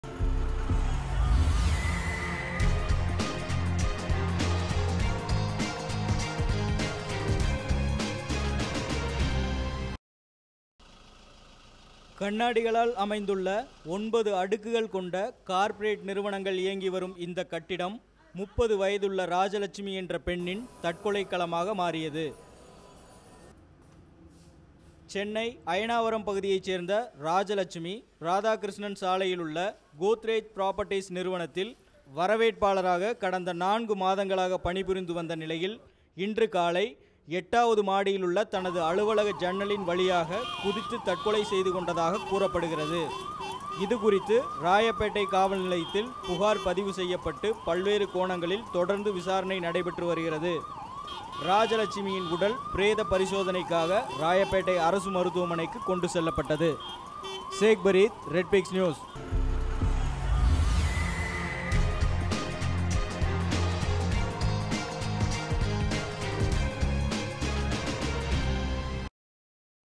Girl commits suicide by Jumping from the 8th floor in Chennai city -- Red Pix
A 25-year-old woman committed suicide by jumping from the eighth floor of a building at Royapettah on Monday morning.

Rajalakshmi, employed as a receptionist at a private company situated in building on R K Salai, took the extreme step at 9.45am, police said. The woman joined the company four months ago.Rajalakshmi's father, Selvam, is an accounts manager with a private firm and mother is a teacher.A security man and a sweeper found her lying in a pool of blood on the ground.Police were inquiring with her parents to find out the reason for her suicide.